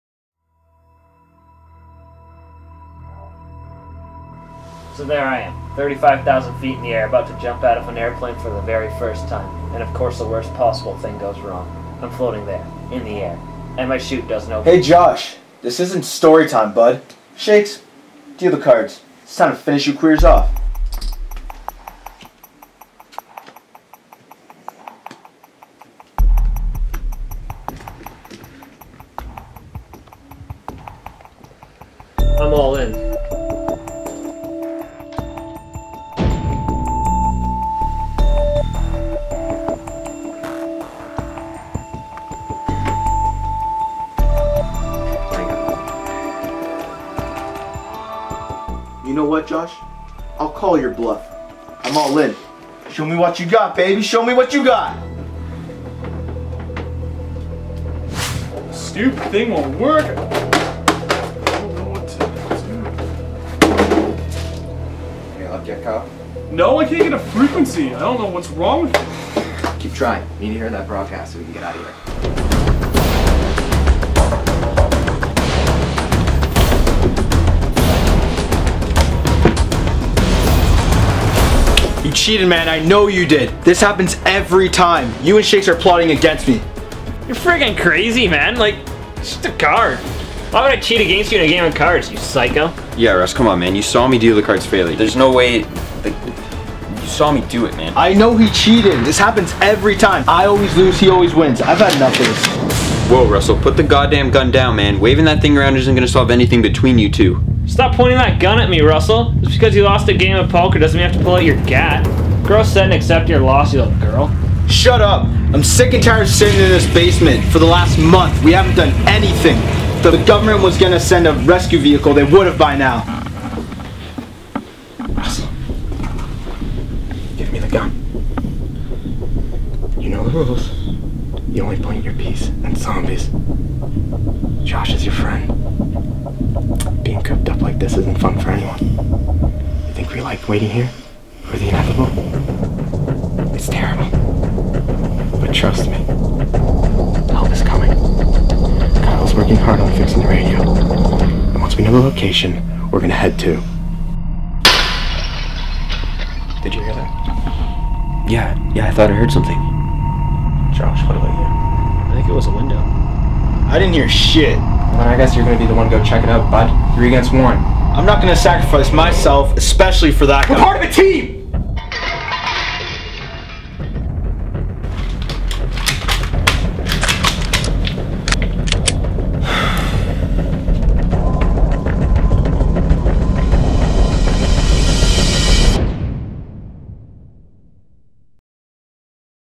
Estallido - PART I
The FIRST episode in a FULL 1080p HD 8-Part mini series about five guys fighting for their lives.

Directed, Produced, Edited, Filmed & Screenplay
by Kyle Popovich

Story
by Justin Doran